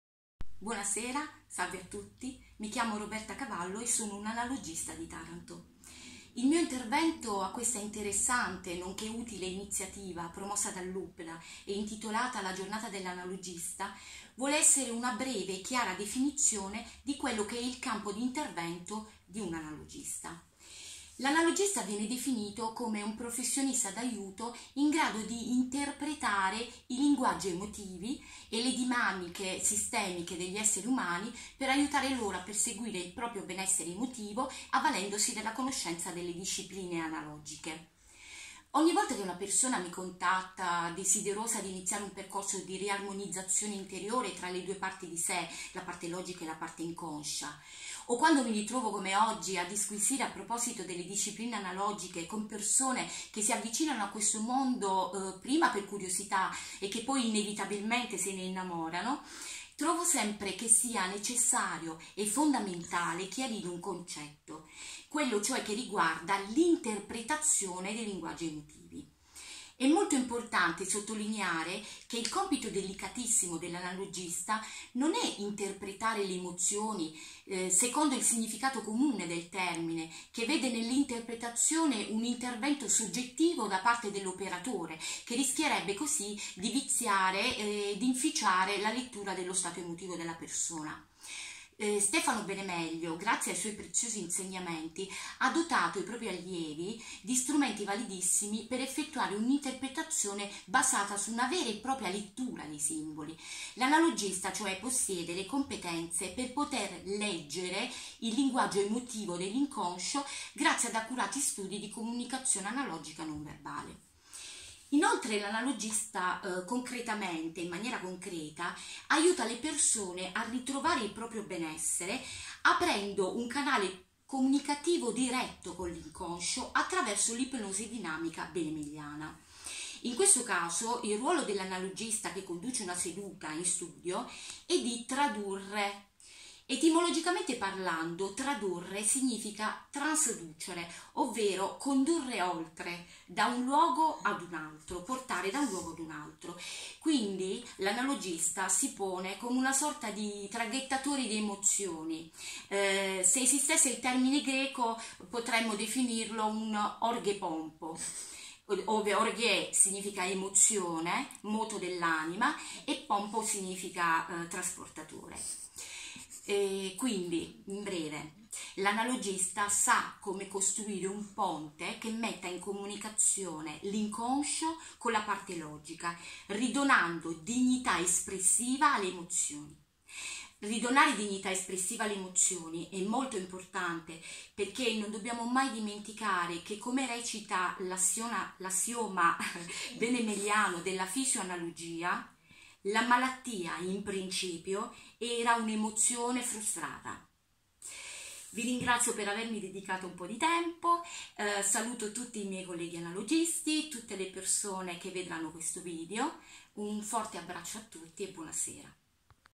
Roberta Cavallo - Analogista
Di cosa si occupa un Analogista?
Pillole di Benessere. Gli Analogisti si raccontano